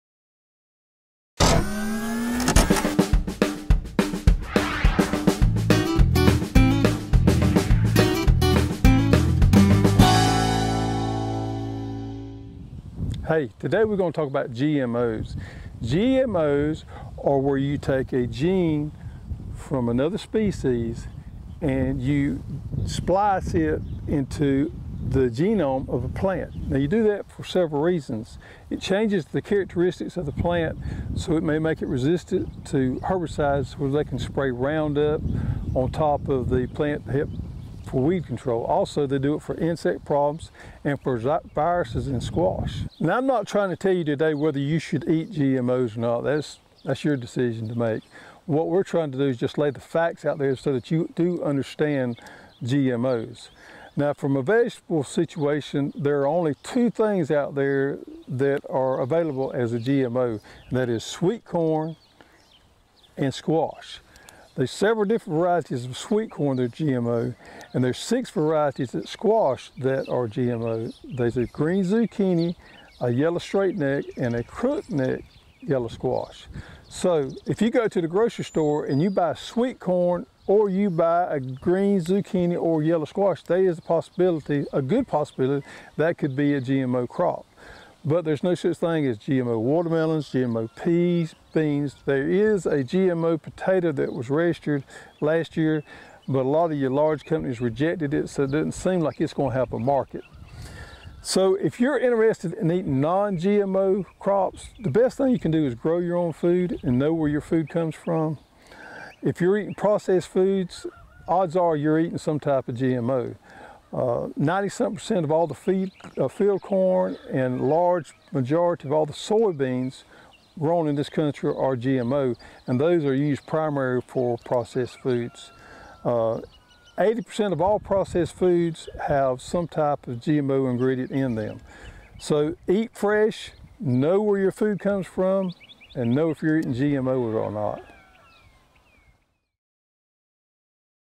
Are GMOs in Your Garden?
Although many of the seed catalogs would have you believe otherwise, the only vegetable crops for which GMO varieties have been developed are sweet corn and squash. These are not readily available to the public, so you don't have to worry about growing them in your vegetable garden.